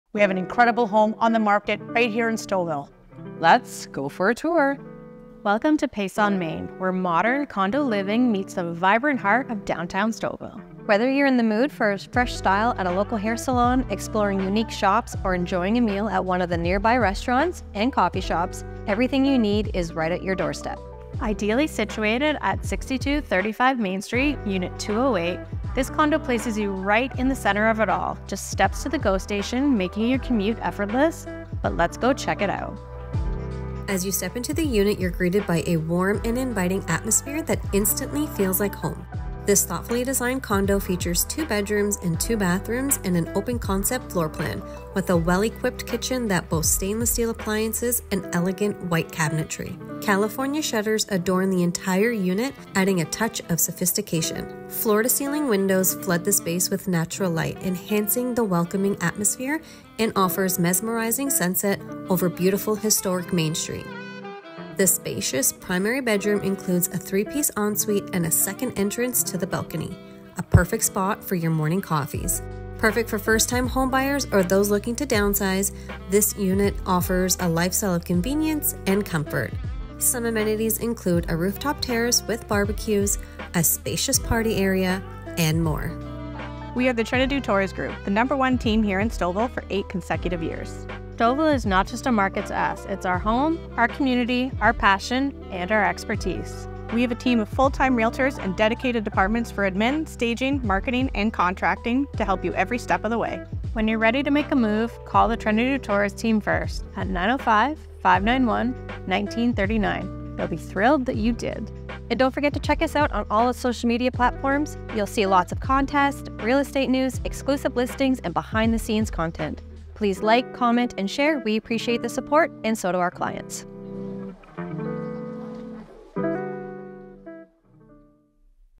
#208-6235 Main St, Stouffville - Presented By The Trentadue Torres Group
📍208-6235 Main St, Stouffville

Experience luxury condo living in the heart of historic Stouffville. This 2-bedroom, 2-bathroom unit is perfect for first-time homebuyers entering the market or downsizers looking for convenience.

Located just steps from daily amenities and the vibrant events that make Stouffville so special, this condo offers the perfect blend of comfort and lifestyle.

Contact Sonya, Dolores, and our award-winning team today to start your journey:

📱 | DM us!
💻 | TTGRealty.com